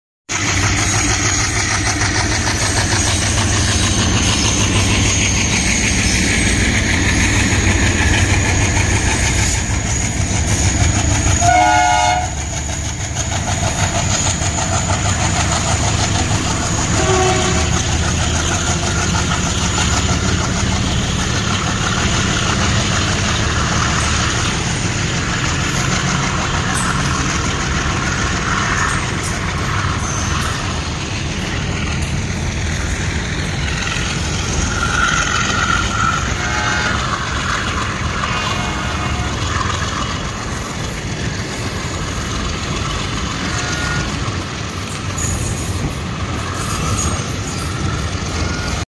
Parallel Diesel Action of Tilak Bridge Shri Ganganagar exp Goods train
Beautiful crossing of Tilak Bridge Shri Ganganagar express with goods train at Kirti Nagar Railway station with melodious chugging sound